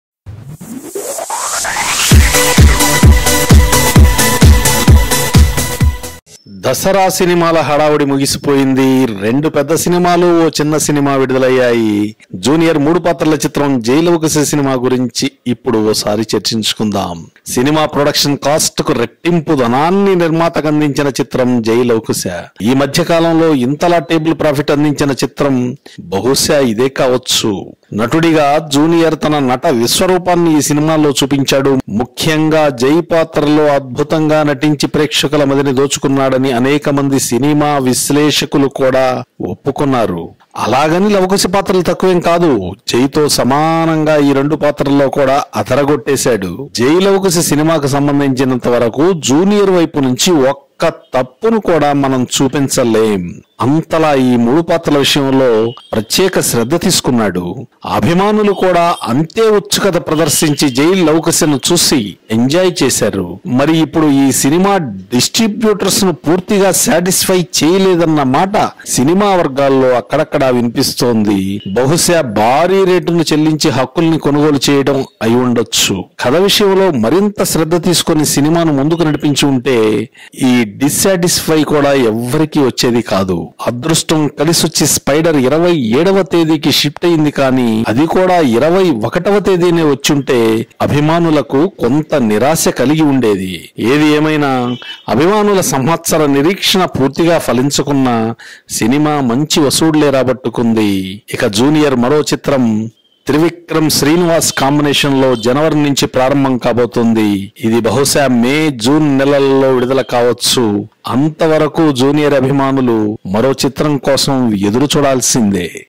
Jai Lava Kusa Movie Distributors Are Not Satisfied With Movie Collections | Jr NTR | Nivetha Thomas
Jai Lava Kusa Movie
Starring Jr NTR , Rashi khanna , Nivetha Thomas And Others In Lead Role
Director : KS Ravindra (Bobby)
Producer : Kalyan Ram
Banner : NTR Arts